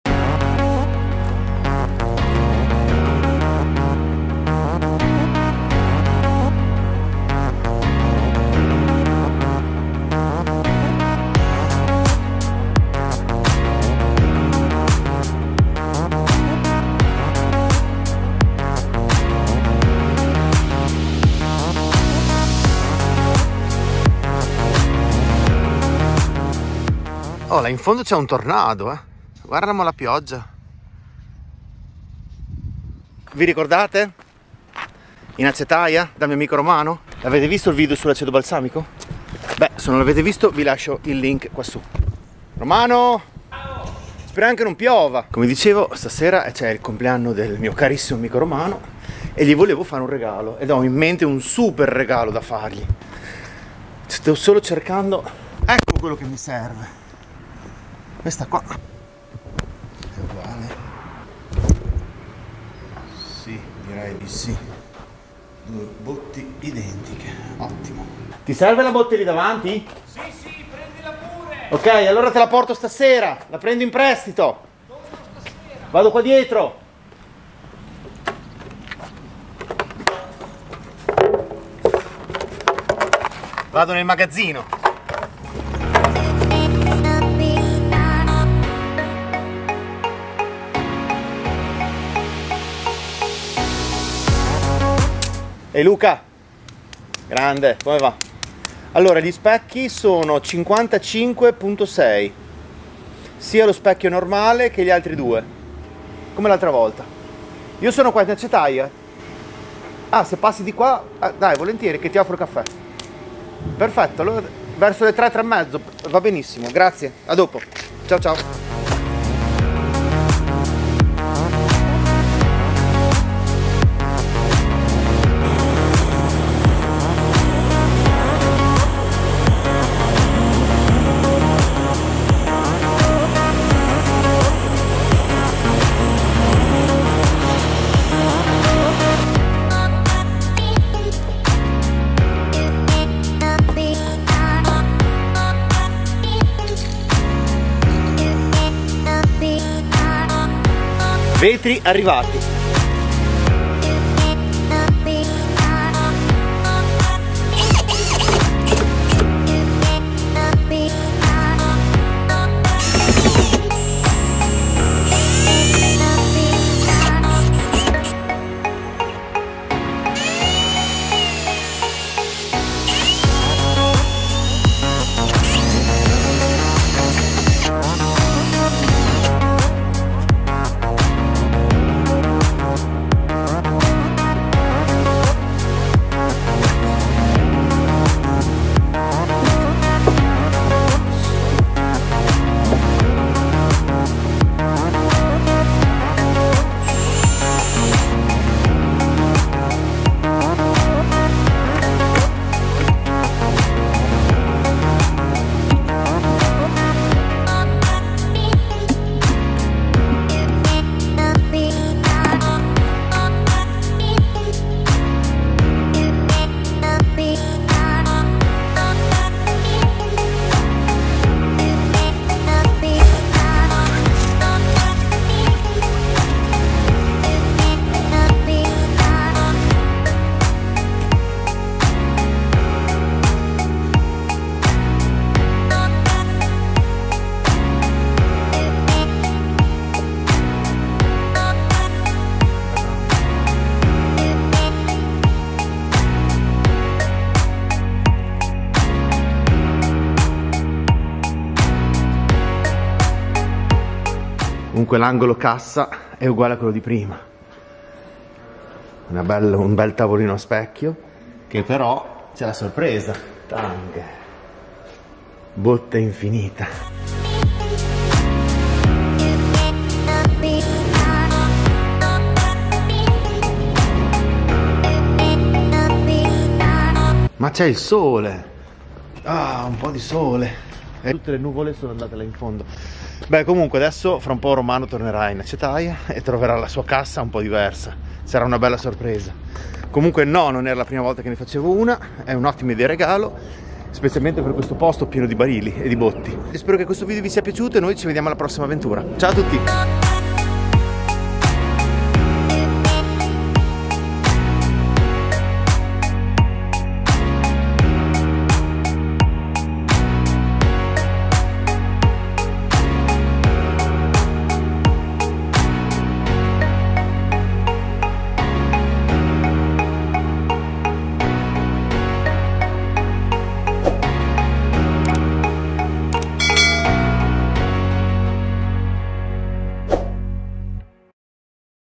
Una botte infinita
Di nuovo in acetaia, se non avete visto il video sull acetoBalsamico di modena guardatelo

Oggi rivoluzioni l angilo cassa con una botte a fondo infinto, un gioco di specchi dall’effetto spettacolare.